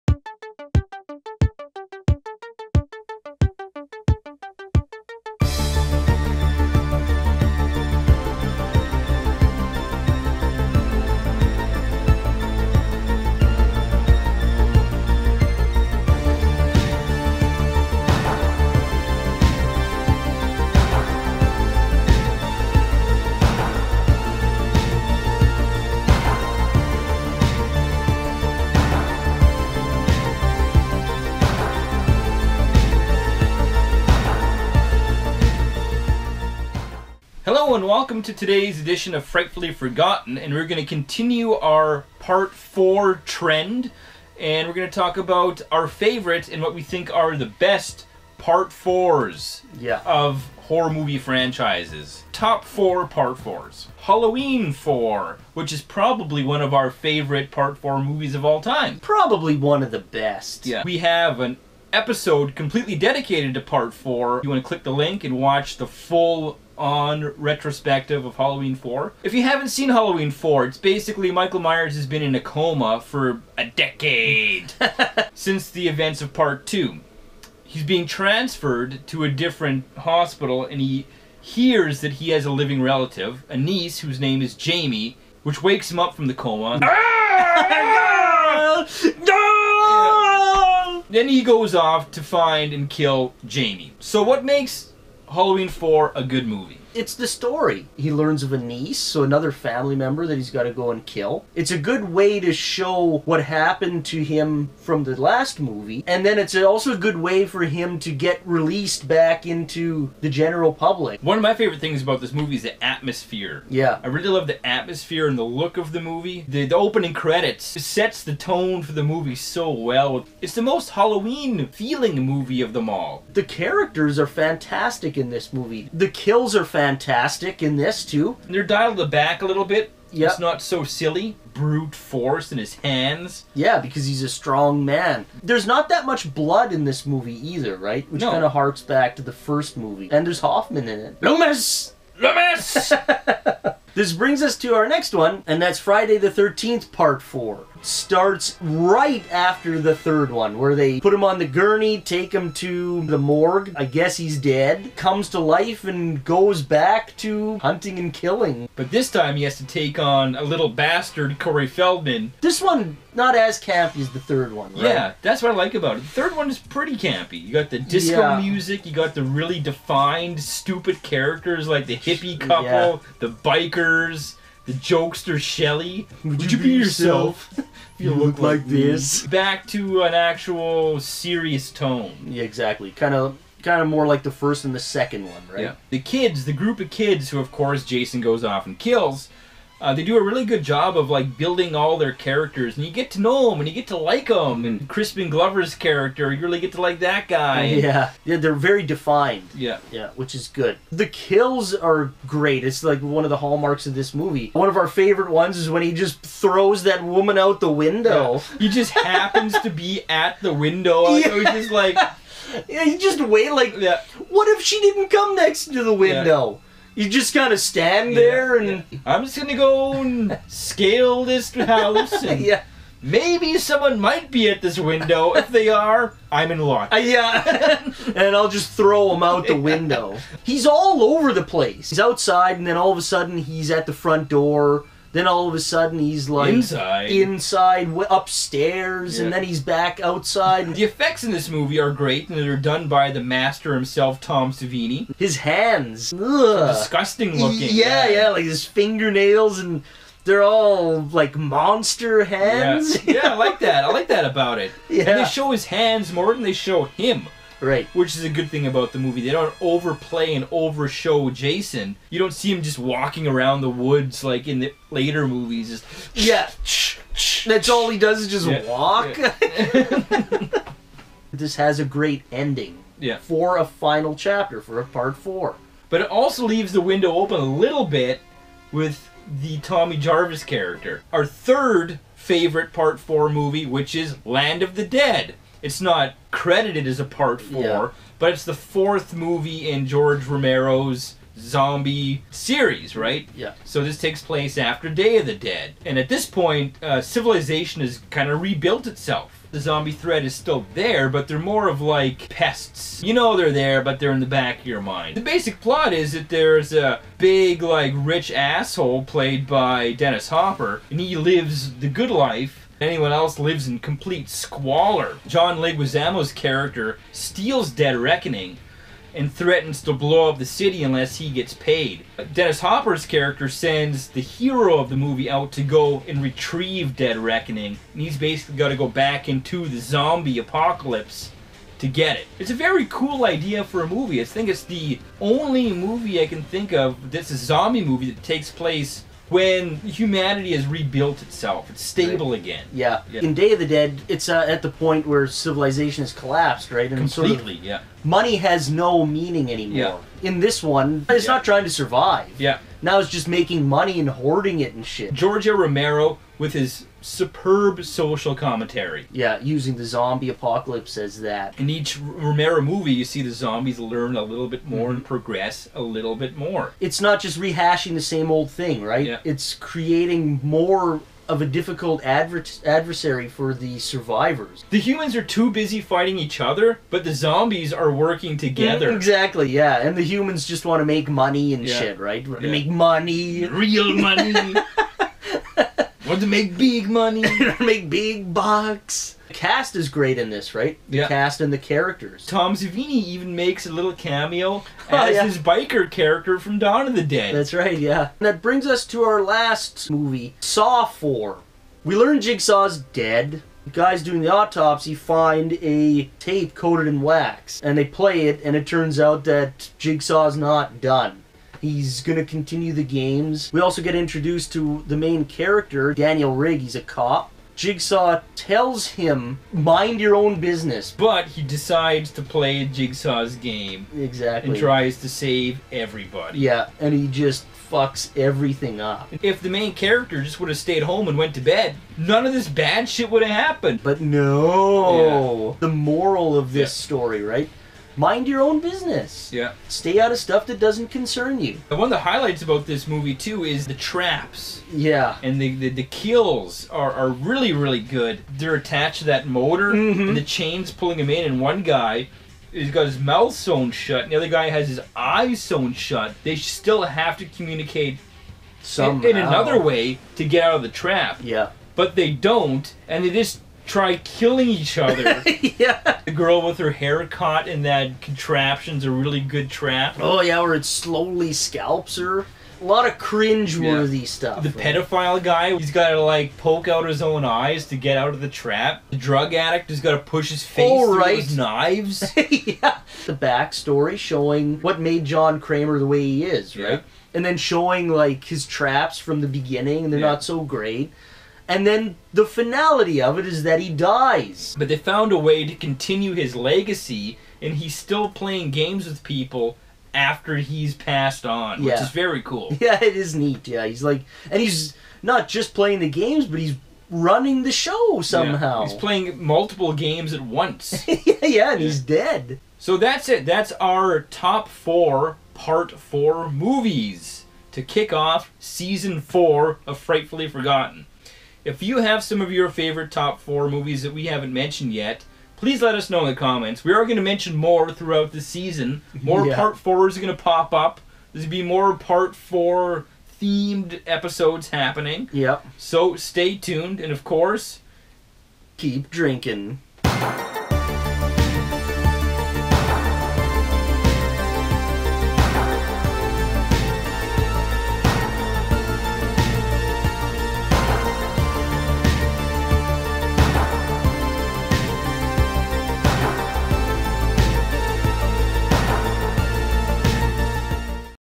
The BEST Part 4 Horror Movies!
To celebrate Season 4 of Frightfully Forgotten it's only fitting to list our top 4 part 4 horror movies!